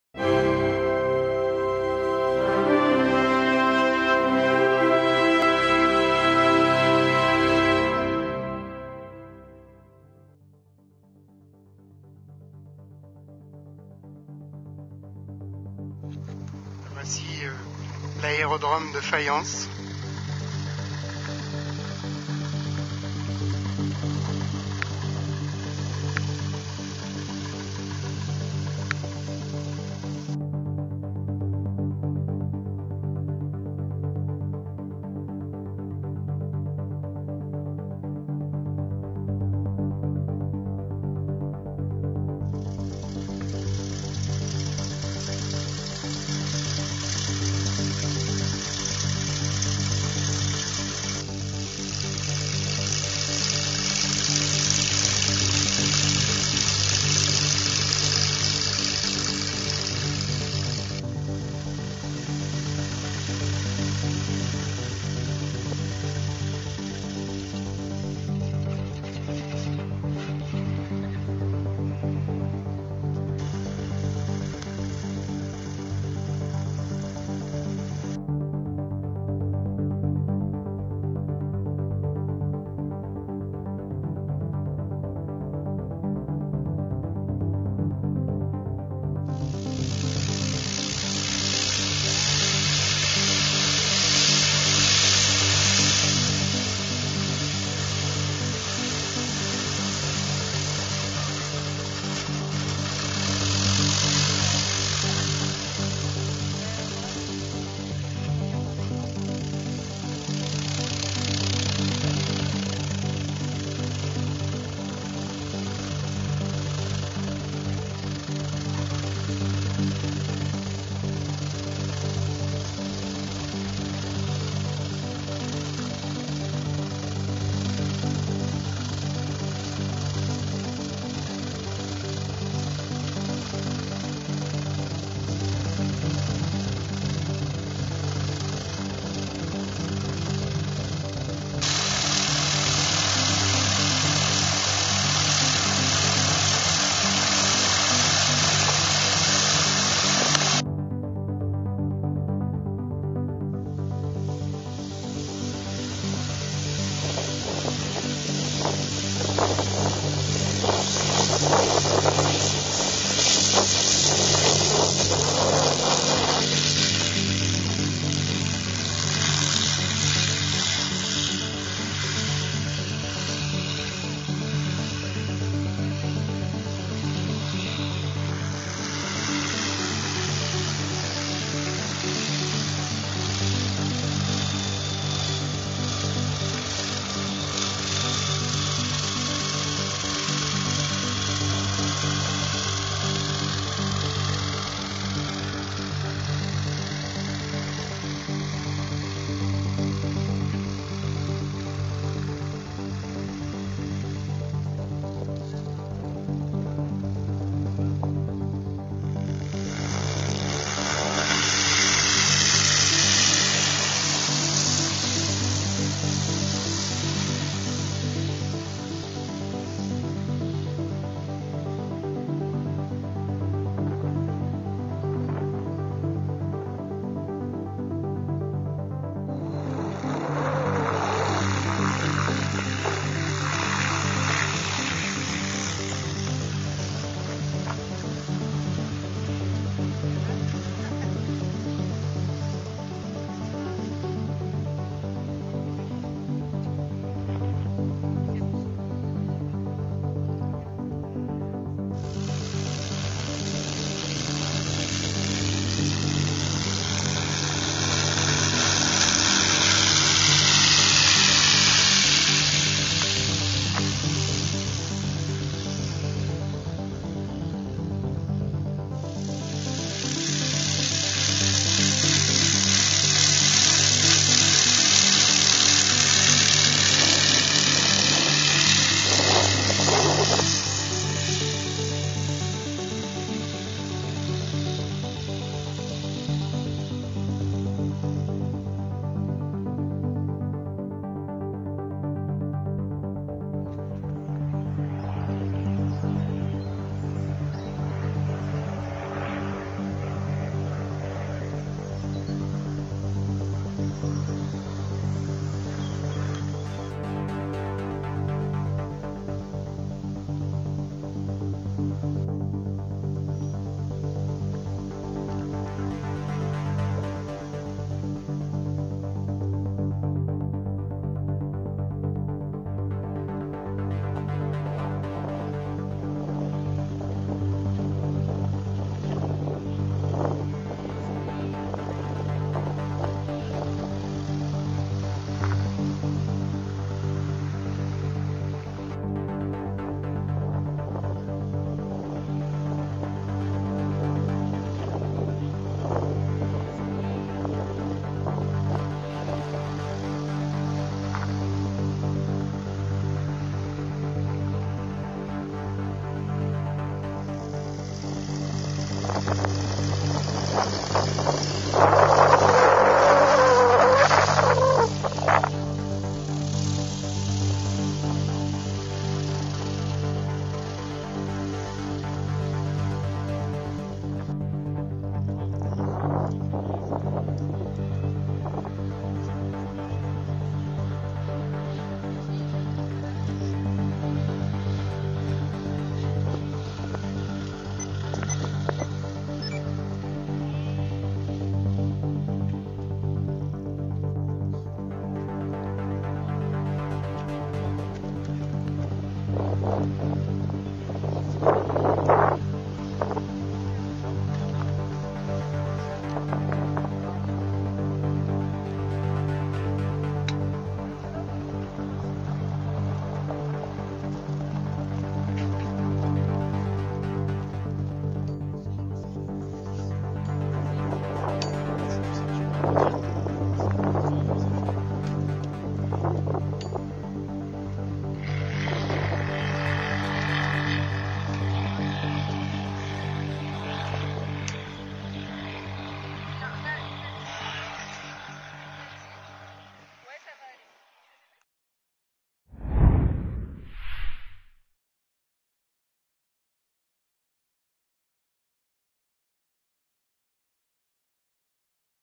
FAYENCE : Le pays du vol à voile - Décollages et atterrissages en direct.
Photos et vidéos : Copyright Gilles Dufour.

Artiste : __url_artiste__

Le vol à voile (ou vol en planeur) est une activité de loisir et de compétition au cours de laquelle ses adeptes appelés vélivoles utilisent un aérodyne appelé planeur.
Alors que certains vélivoles apprécient la liberté, le plaisir esthétique et la joie de maîtriser leurs machines, d'autres se concentrent sur leur performance en compétitions. Ces concours confrontent les capacités des pilotes et copilotes (dans le cas de machines biplaces) à reconnaître et utiliser les conditions aérologiques ainsi que leurs qualités de pilotage et de navigation. Il existe enfin la pratique de la voltige aérienne en planeur.
En compétition, les parcours peuvent être de différents types :
• soit à buts fixés dans un minimum de temps ;
• soit un circuit libre où seule la distance est prise en compte ;
• soit sur la plus longue distance possible pour une durée fixée.
Qu'elle soit de loisir ou de compétition sur circuit, la pratique du vol à voile consiste à exploiter au mieux les courants aériens ascendants ou « ascendances ».